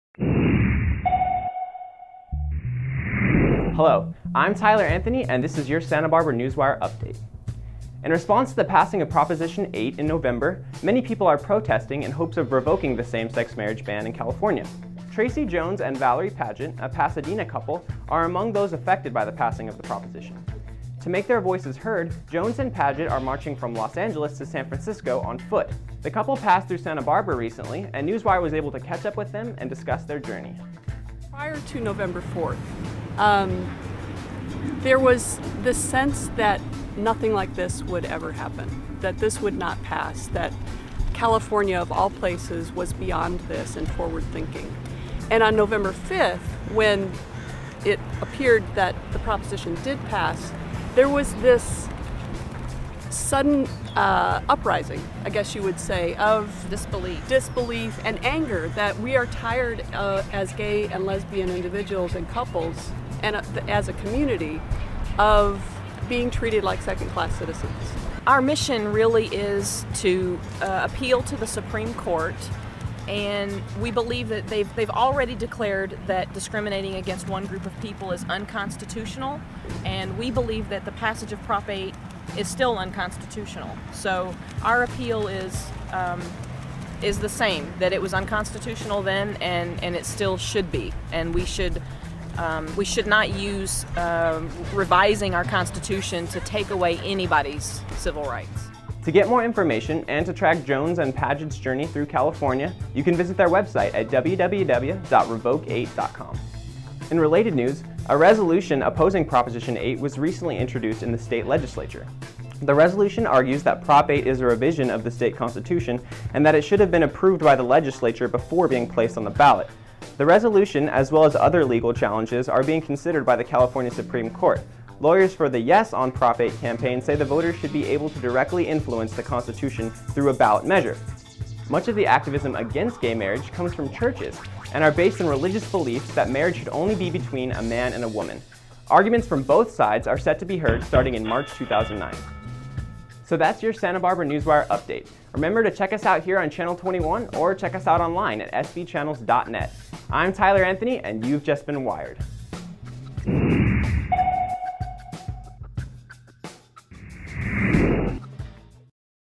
SB Newswire 12.05.08 - Vintage TVSB
Our intern Tyler Anthony gets you "wired" on the heated battle regarding same sex marriage and one couples walk across California to bring social awareness to repeal Prop.8.
*First published on Dec 5, 2008*